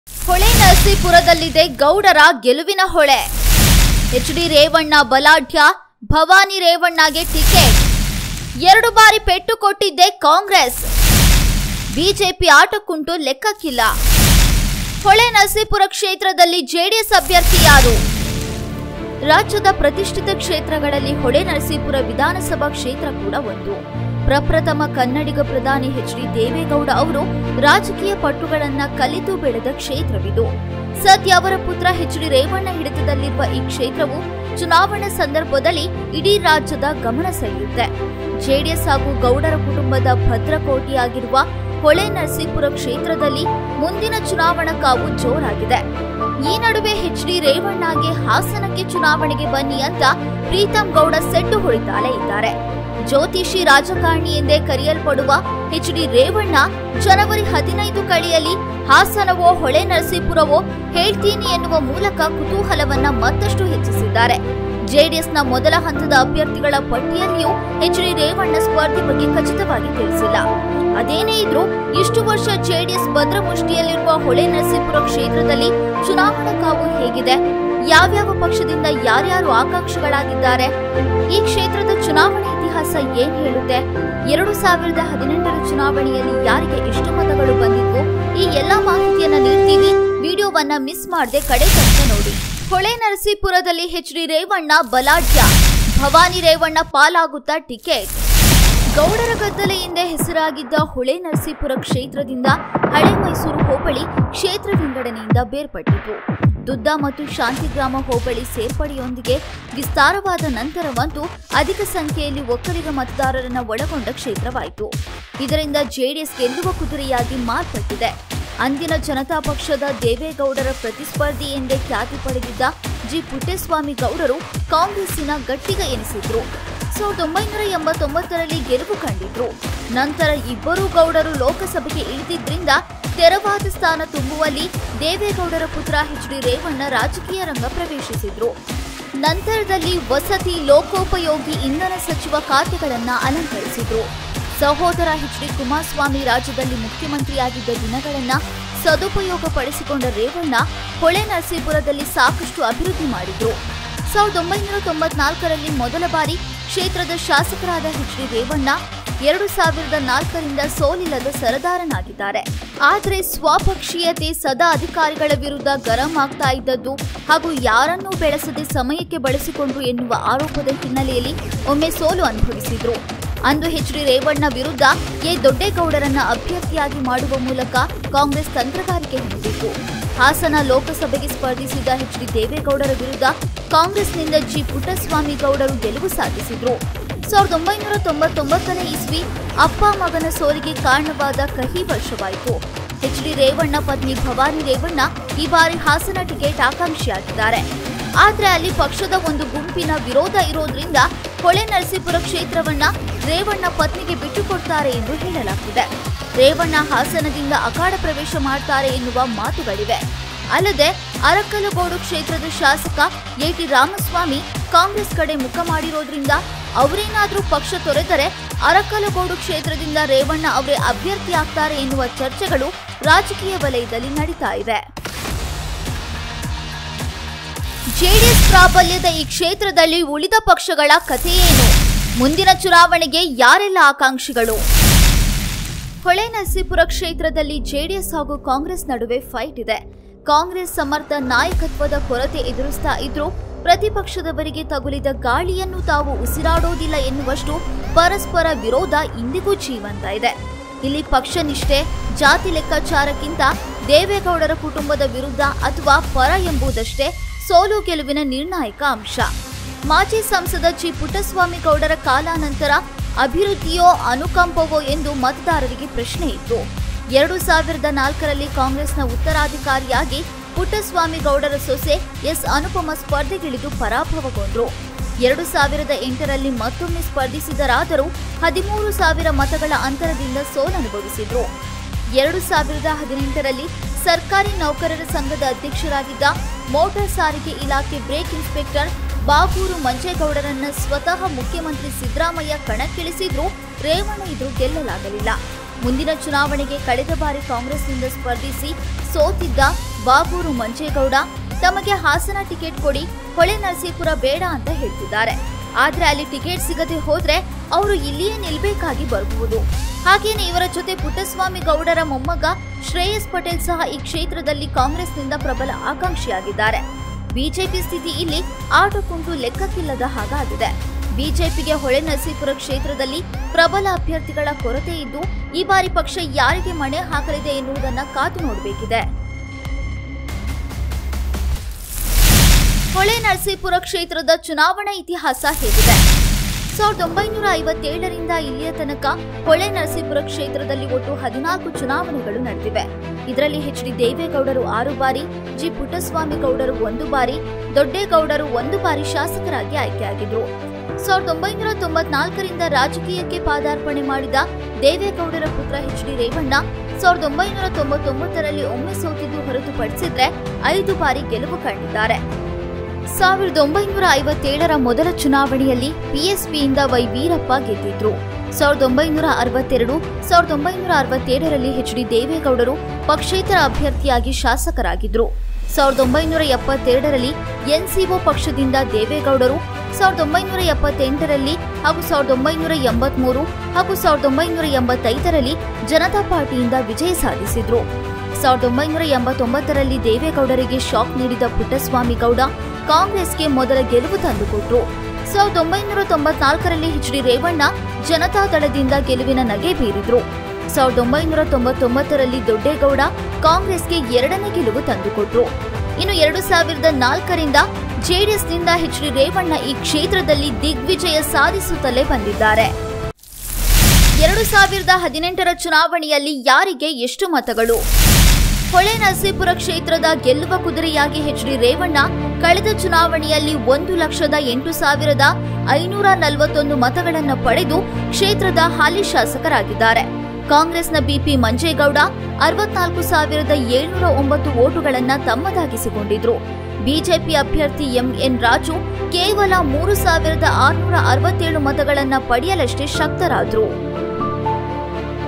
ಹೊಳೆನರಸೀಪುರ ಕ್ಷೇತ್ರದಲ್ಲಿ JDS ಅಭ್ಯರ್ಥಿ ಯಾರು.? | HD Revanna ಬಲಾಢ್ಯ | Holenarasipur Constituency
ಹೆಚ್.ಡಿ. ರೇವಣ್ಣ ಬಲಾಢ್ಯ
ಭವಾನಿ ರೇವಣ್ಣಗೆ ಟಿಕೆಟ್..!?
ಎರಡು ಬಾರಿ ಪೆಟ್ಟು ಕೊಟ್ಟಿದೆ ಕಾಂಗ್ರೆಸ್
ಬಿಜೆಪಿ ಆಟಕ್ಕುಂಟು ಲೆಕ್ಕಕ್ಕಿಲ್ಲ